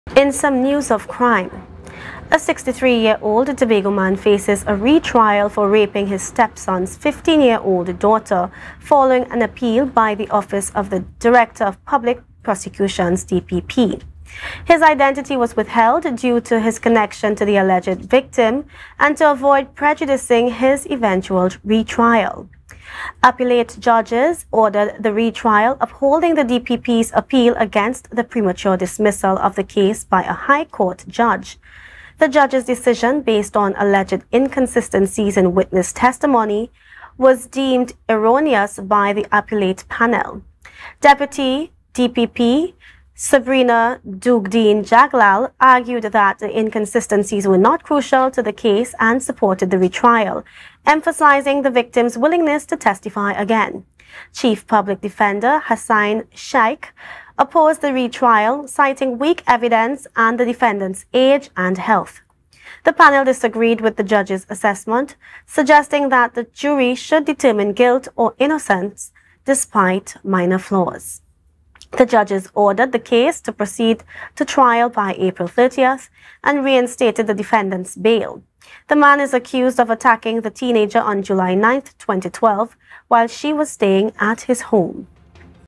63 YEAR OLD FACES RETRIAL FOR RAPE
A 63-year-old Tobago man faces a retrial for raping his stepson’s 15-year-old daughter, following an appeal by the Office of the Director of Public Prosecutions (DPP).  His identity was withheld due to his connection to the alleged victim and to avoid prejudicing his eventual retrial. Appellate judges ordered the retrial, upholding the DPP's appeal against the premature dismissal of the case by a High Court Judge.
tobagoupdatesnews | WATCH LIVE AT 6:30PM | AMPLIA CHANNEL 102